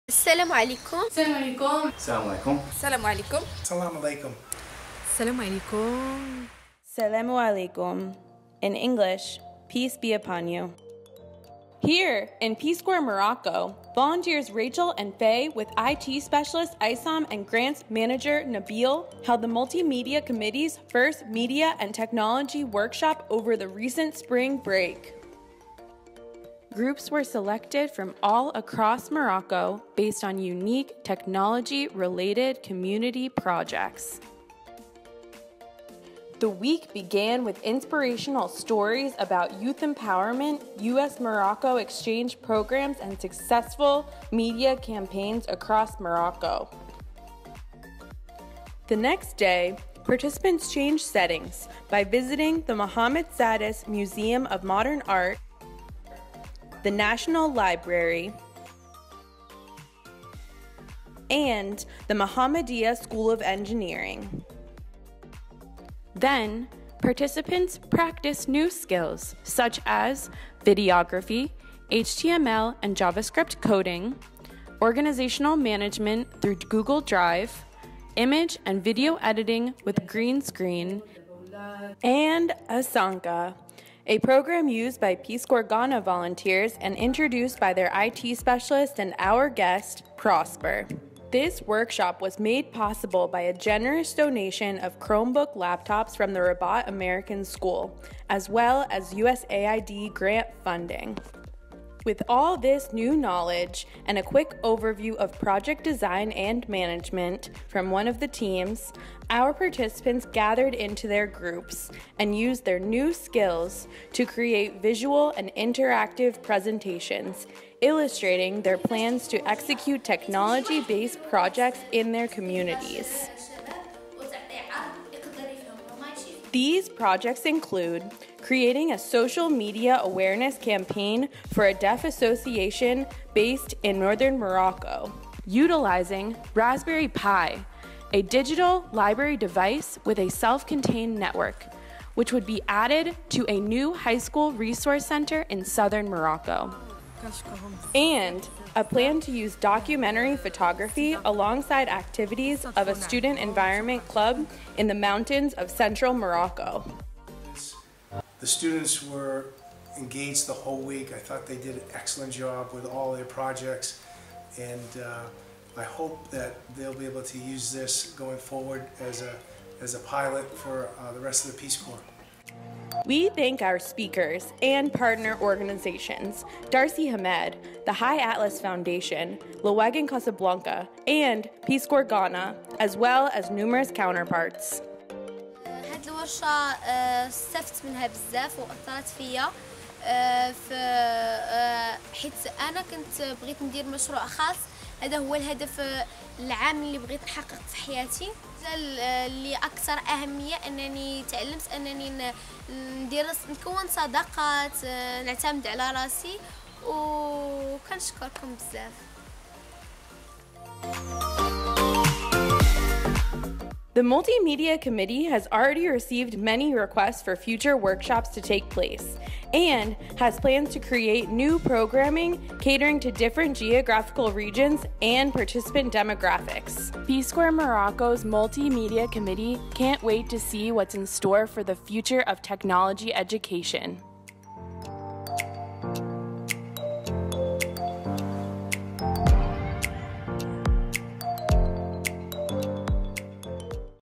Media & Technology Workshop 2019
Through the Media & Technology Workshop for Youth Engagement in Rabat, fifteen Moroccan youth and counterparts from five Peace Corps Volunteer (PCV) sites planned community development projects incorporating media and technology skills to be implemented in their host communities. Participants were inclusive of gender, age, special needs, skill level, and geographical diversity.

The Media & Technology Workshop 2019 was created and developed by Rachael Diniega, Fay Cowper, Aissam Talouni, and Nabil El Asri

Video created by Fay Cowper

Arabic Translations by Hasna Es-sayid

من خلال ورشة " الإعلام والتكنولوجيا لمشاركة الشباب " التي أقيمت في الرباط ، قام خمسة عشر شابًا مغربًيا ونظيرًا من خمسة مواقع متطوعي هيئة السلام بتخطيط مشاريع تنمية المجتمع متضمنة لمهارات الإعلام والتكنولوجيا التي سيتم تنفيذها في المجتمعات المضيفة. تم إشراك المشاركين بنوع الجنس، العمر،الاحتياجات الخاصة ،مستوى المهارة والتنوع الجغرافي.
تم إحداث و تطوير ورشة الإعلام و التكنولوجيا 2019 من طرف رايتشل دينيجا، فاي كوبر، عصام تالوني و نبيل العسري
الفيديو تم إنشاءه من طرف فاي كوبر
الترجمة إلى العربية: حسناء السَّيد